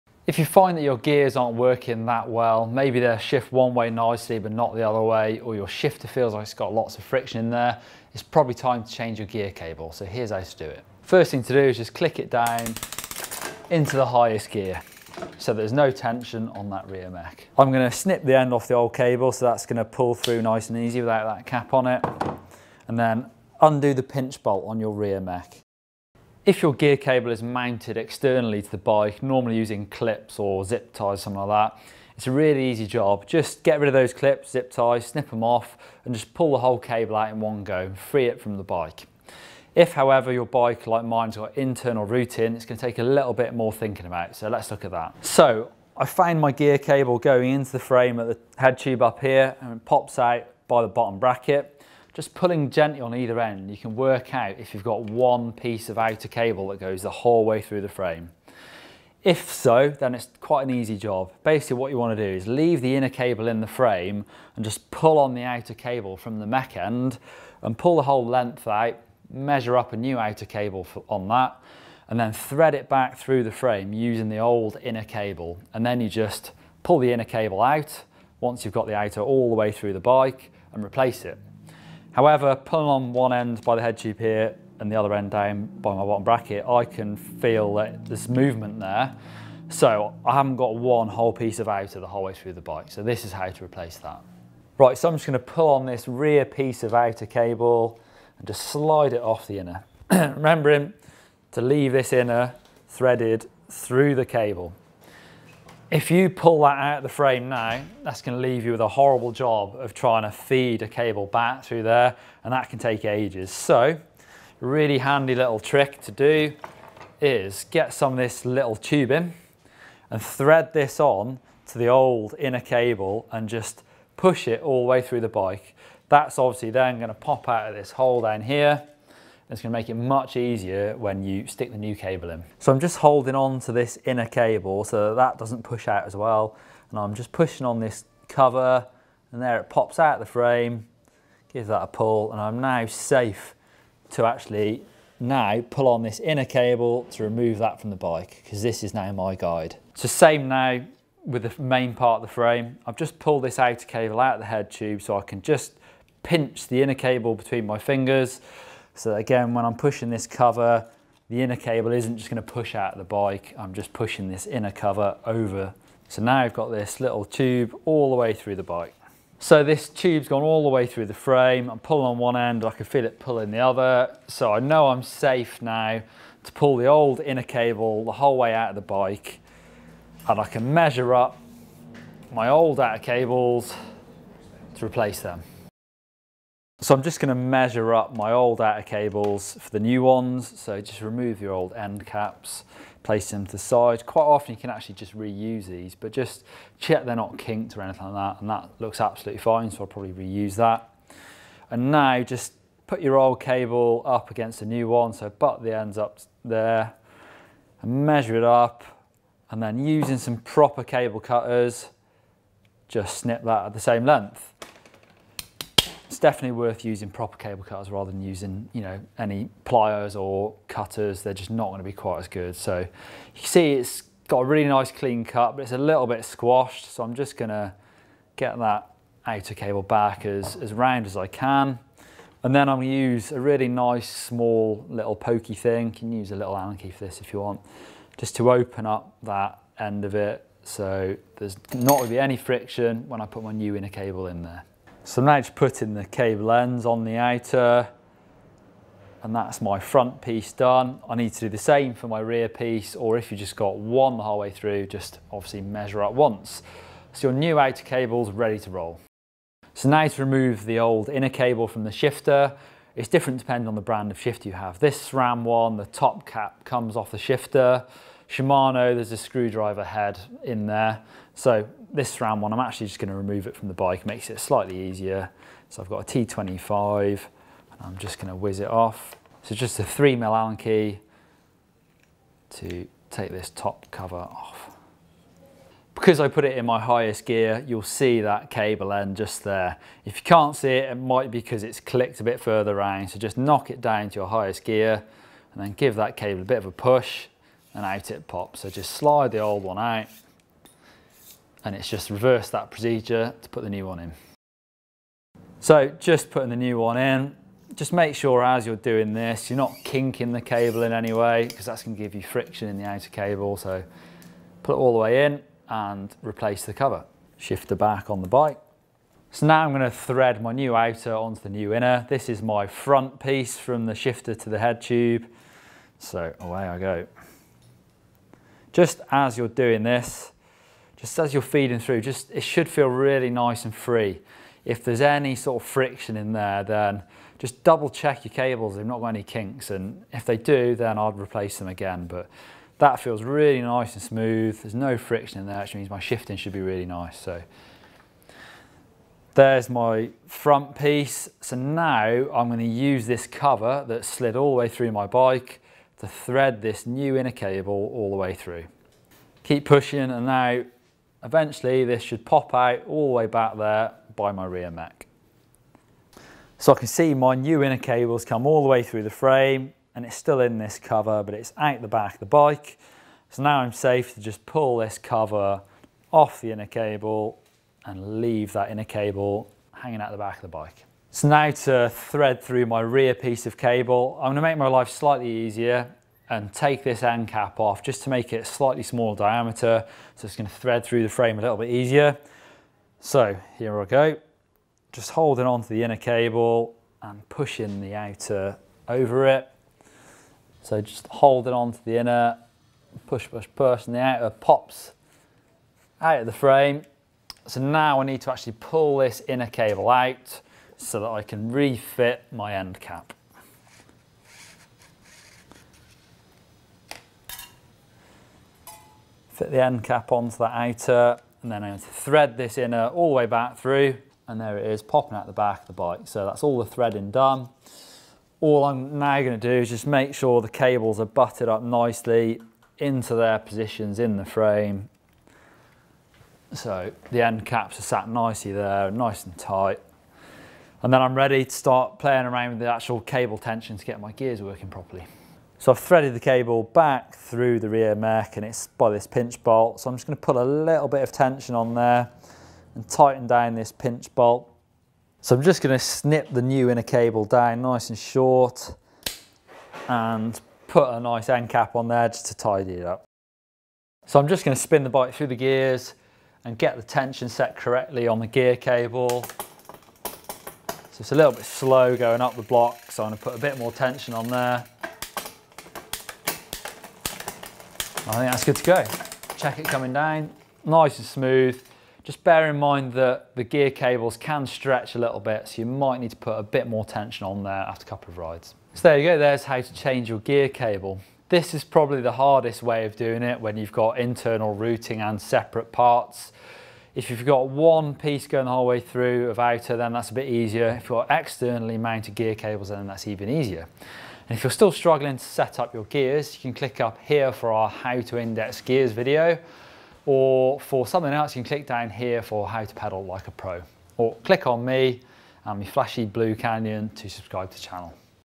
How To Replace Gear Cables - Mountain Bike Maintenance
Are your gears not shifting like they used to? It's time to replace your cables!

After pumping up your tyres, replacing cables is probably the 2nd most common maintenance job on your bike. Although it can be made a bit trickier with the advent of internal cable routing on some newer bikes, it requires just an allen key, a set of replacement cables and a good set of cable cutters to get the job done.

Replace your inner cables any time they look rusty, worn or frayed. If it feels like the cables are coming up against a lot of friction and the inner cables are ok, then it might be time to replace the plastic outer housing too. Neil shows you how to do the complete job, however your bike happens to be set up.